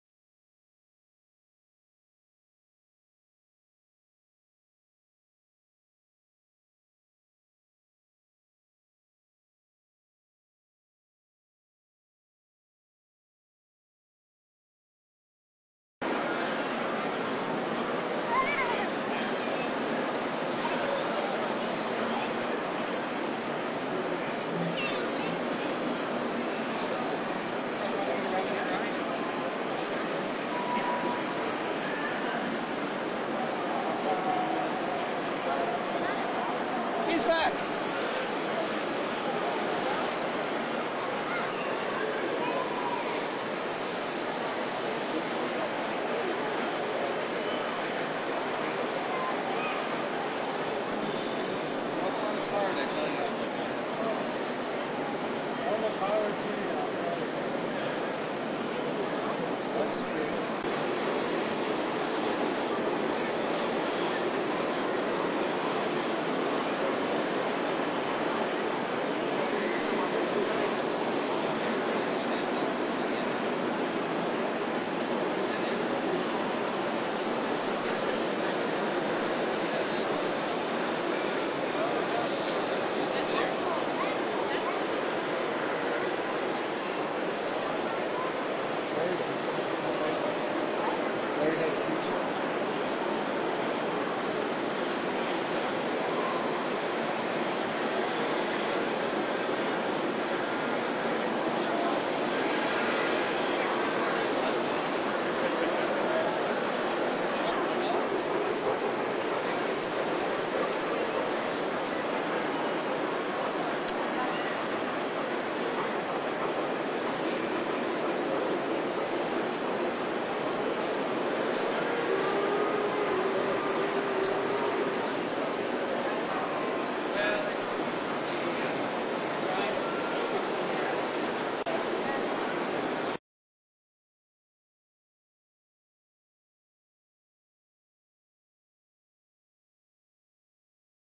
"Wildwood" N Scale Model Train Layout 2008
This is an N scale model train layout featured at SuperTrain, 2008, held at the Calgary Soccer Centre, April 19-20th. This beautiful layout represents the English country side, around 1906 and features 3 independent rail lines.  Michael Willis of Calgary is the owner of this masterpeice. Great job Mike! I was fortunate enough to assist in operating this fine layout for a few hours during the show.